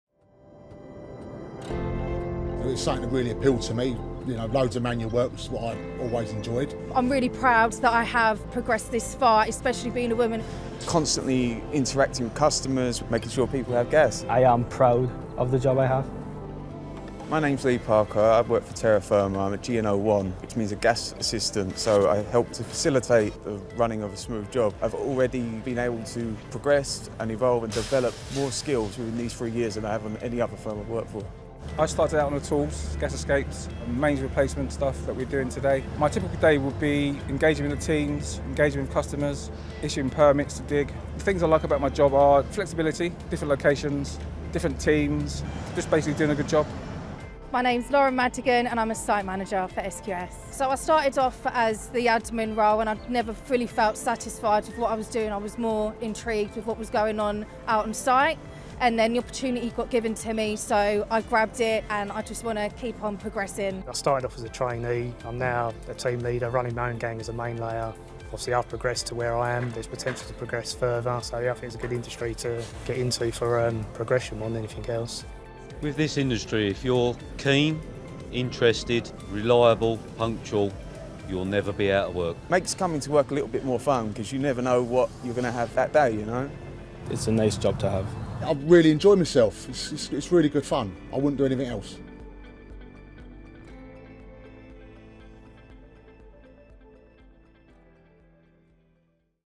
Cadent Recruitment Advert - North London September 2022
Do a job that matters - discover a job that makes a difference to millions of people in their homes.

The jobs available with Cadent partners are varied and are open to people with no experience to supervisors.  If you are unsure of your career path or you want to change your career and think engineering could be something you are interested in, then contact one of these companies today.  Do a job that matters and see where your career can go.

Importantly, training and qualifications are provided.